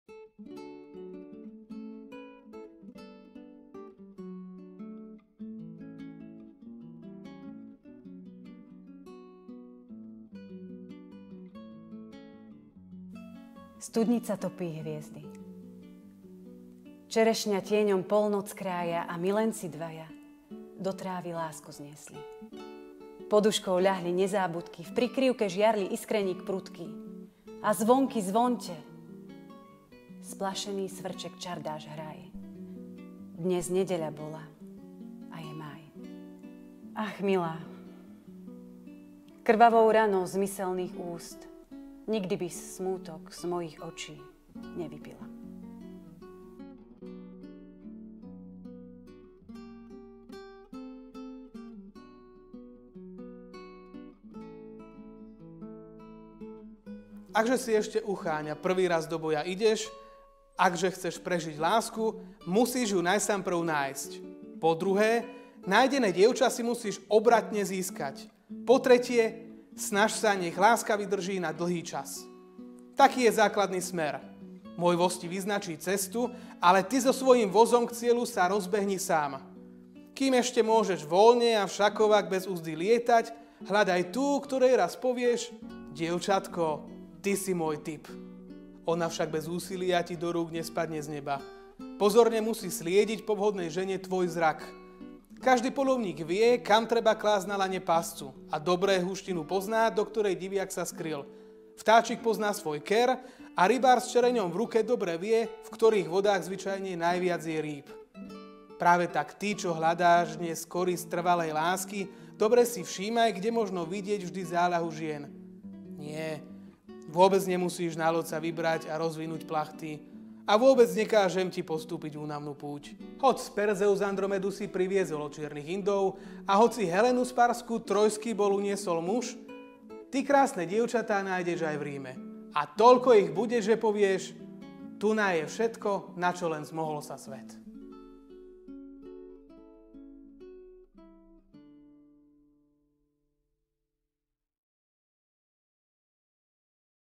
Valentínska čítačka I.
Od dnes spúšťame týždňový maratón ľúbostnej poézie - VALENTÍNSKU ČÍTAČKU z tvorby slovenských aj svetových klasikov. Pozerajte, počúvajte... nielen zaľúbené básne. V I. časti recitujú Miroslava Kupčuláková a Erik Vrábel.